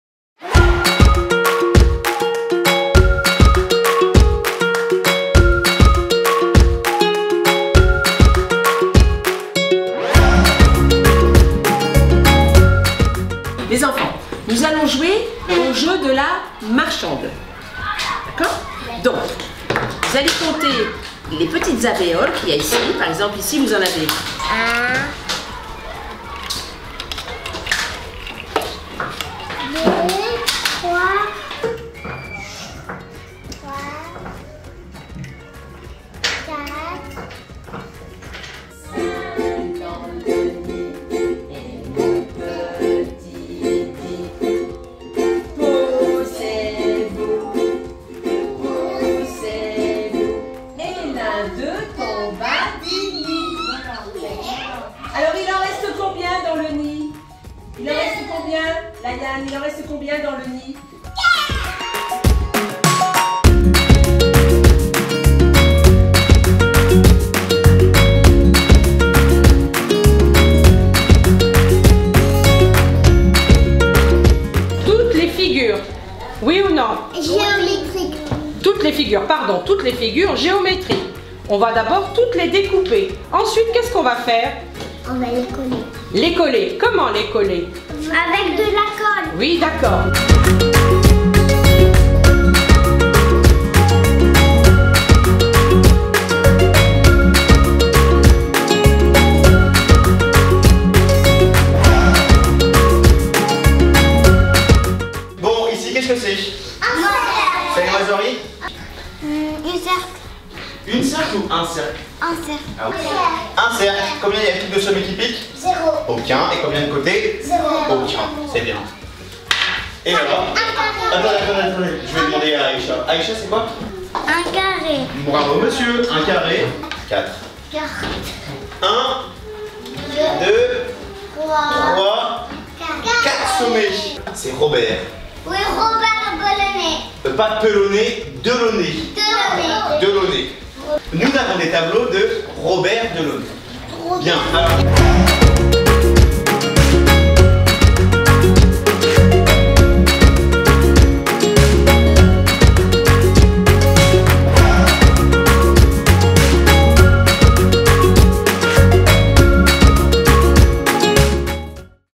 la Semaine des mathématiques TPS- PS, MS et GS
Dans le cadre de la 13e la Semaine des mathématiques, les classes de maternelle (TPS- PS, MS et GS)👧🧒, ont travaillé sur les formes géométriques et leurs caractéristiques.📐📏✏️✂️
Ils ont participé à plusieurs ateliers portant sur la décomposition du nombre, les algorithmes et le jeu de la marchande.🥰🥰
Pour clore la semaine, les élèves ont récité des comptines mathématiques.➕➖➗✖️🟰
AEFE - Agence pour l'enseignement français à l'étranger